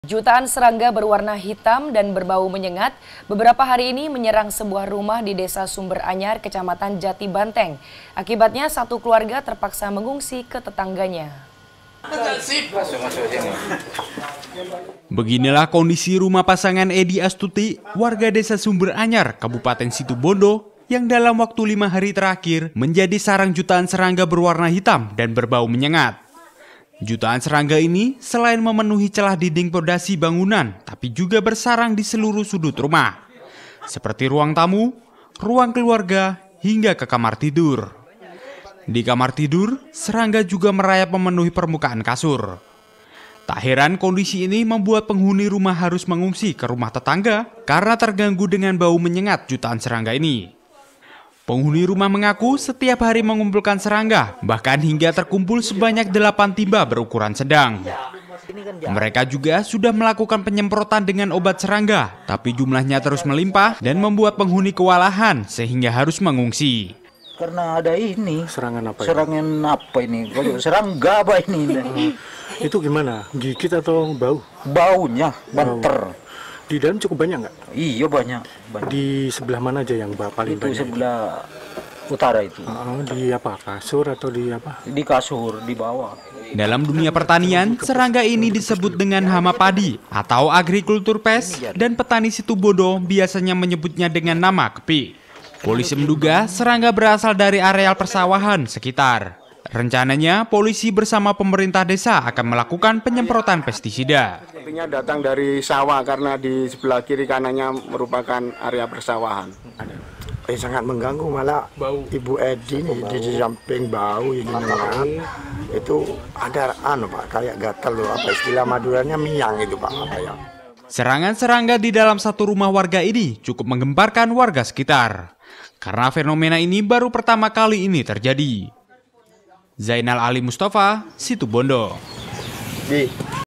Ngeri! Jutaan Serangga Serang Rumah Warga
Jutaan Serangga Berwarna Hitam Dan Berbau Menyengat Menyerang Permukiman Warga Di Desa Sumberanyar Kecamatan Jatibanteng, Situbondo. Kejadian Ini Membuat Satu Keluarga Terpaksa Harus Meninggalkan Rumahnya Mengungsi Ke Tetangga, Karena Hampir Seluruh Ruangan Rumahnya Dipenuhi Oleh Serangga Tersebut.